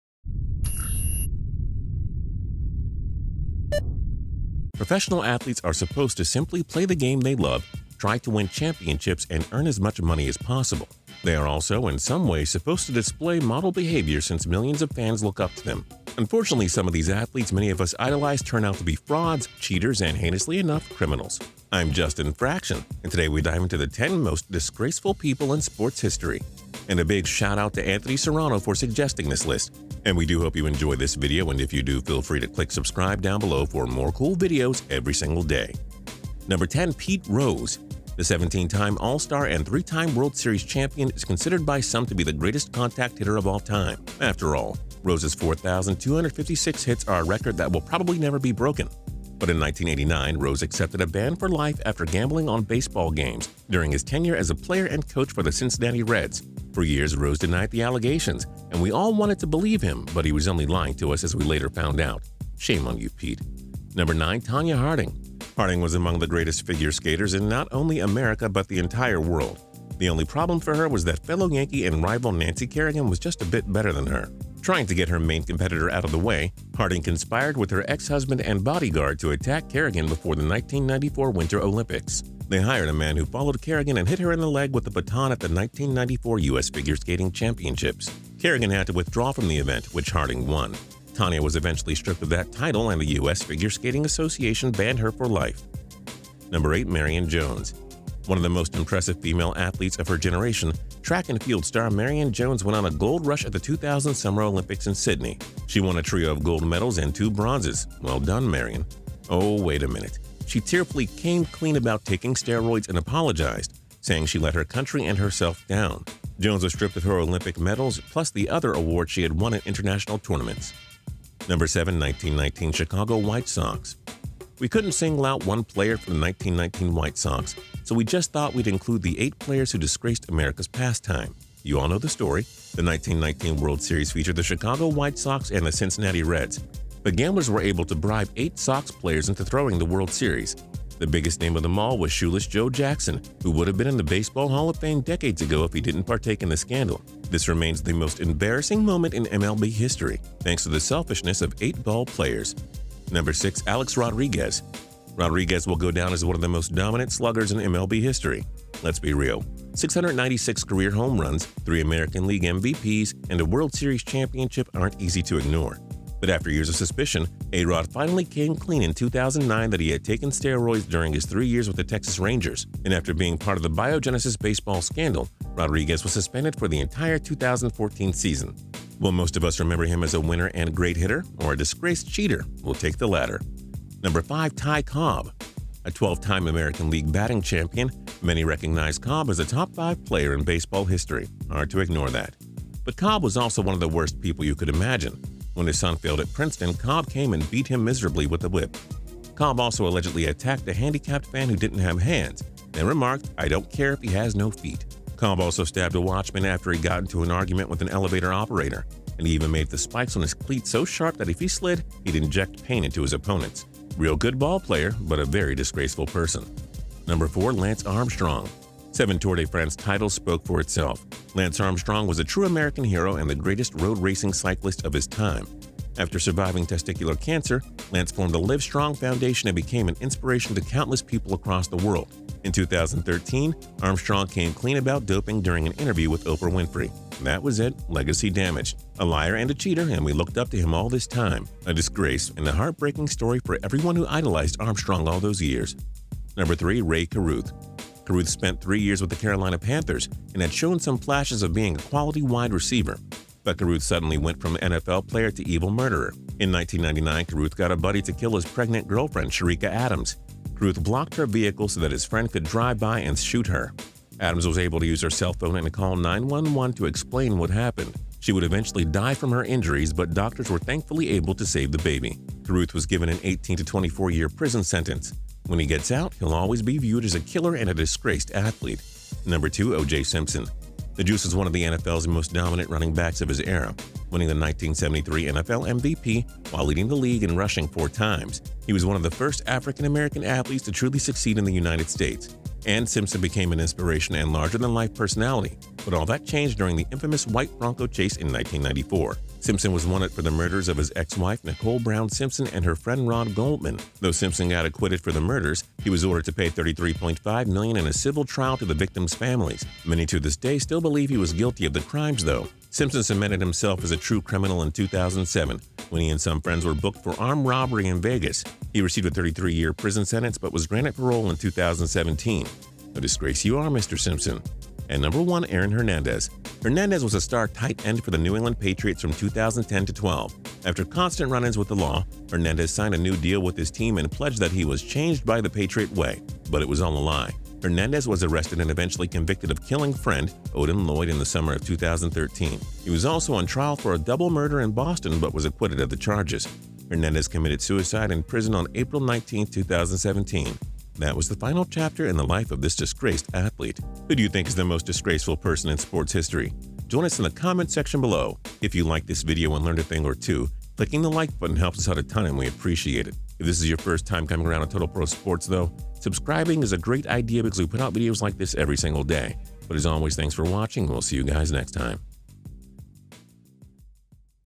10 Most DISGRACEFUL People In Sports History
10 players that disgraced the game

Who do you think is the most disgraceful person in sports history?

Any other Sports lists you want us to do?

Description:
Professional athletes are supposed to simply play the game they love, try to win championships and earn as much money as possible. They are also, in some ways, supposed to display model behaviour since millions of fans look up to them.

Unfortunately, some of these athletes many of us idolized turn out to be frauds, cheaters, and, heinously enough,...criminals. I’m Justin Fraction, and today we dive into the 10 most disgraceful people in sports history.

And a big shoutout to Anthony Serano for suggesting this video!

Video Editor: Scott Shawver
Voice Over: Justin Fraction
Written By: Alex Hoegler